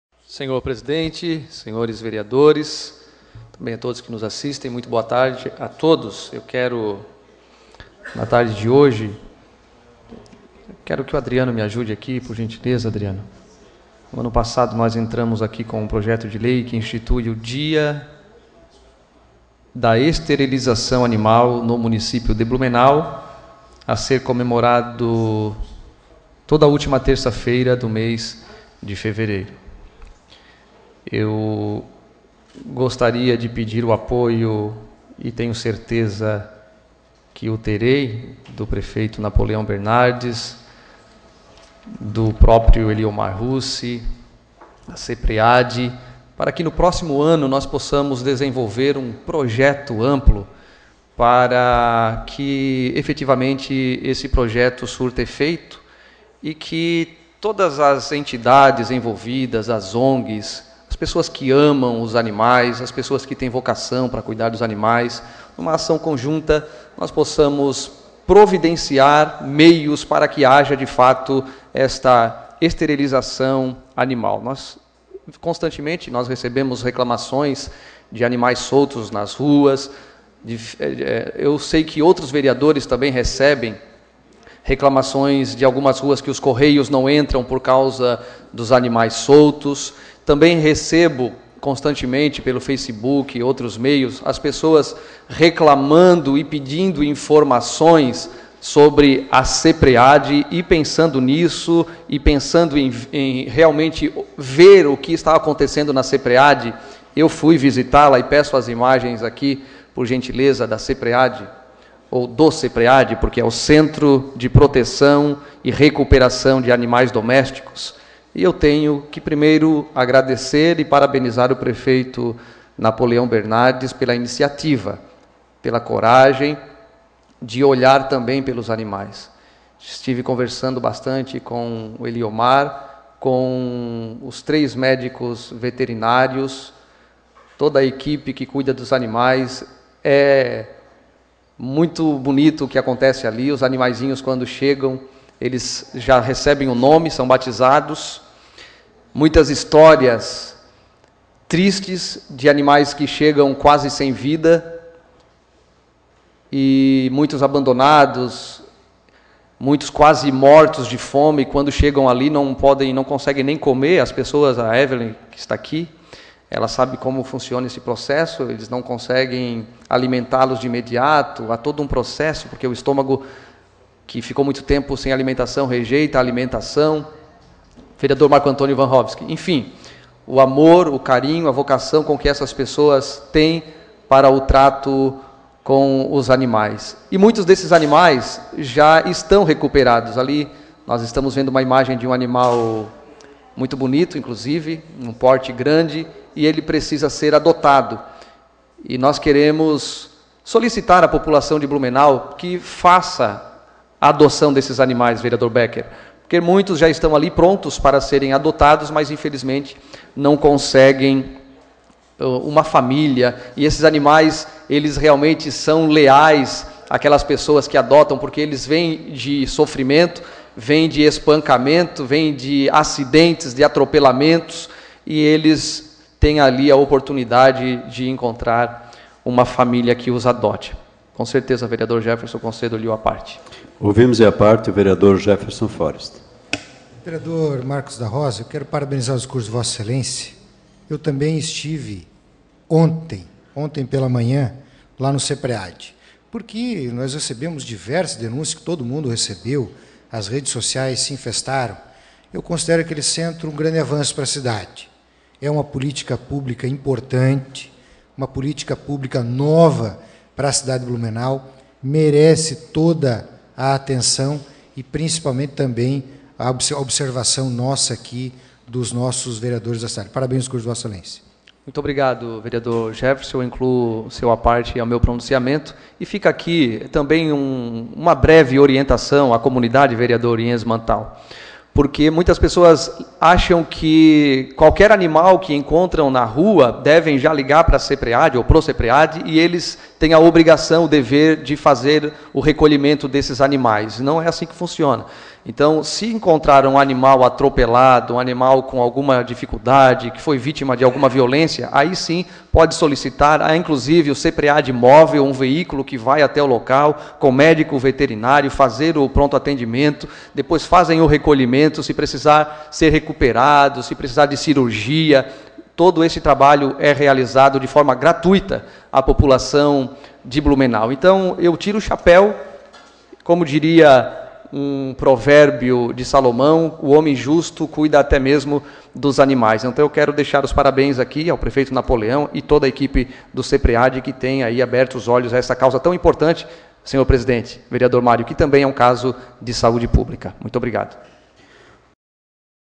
Pronunciamento Vereador Marcos da Rosa - 24/02/2015
Vereador Marcos da Rosa fala sobre o trabalho desenvolvido pelo Cepread
O vereador Marcos da Rosa (DEM) relembrou que é um dos autores do projeto de Lei aprovado no ano passado que institui o Dia Municipal de Esterilização Animal na última terça-feira do mês de fevereiro. Pediu o apoio do prefeito Napoleão Bernardes (PMDB) e do responsável pela Diretoria de Bem Estar Animal, Eliomar Russi, para que no próximo ano haja um projeto amplo em conjunto com ONGs para que a lei seja aplicada e haja uma esterilização em massa de animais. Relatou que esteve visitando o Cepread e parabenizou o trabalho que é desenvolvido na recuperação de animais. Pediu que a população possa adotar os animais que já receberam tratamento. Por fim, frisou que o Cepread não tem obrigação de recolher animais das ruas, mas se propõe a atender e recuperar animais de rua com problemas de saúde.

Foto: Renan Olaz | Agência CMB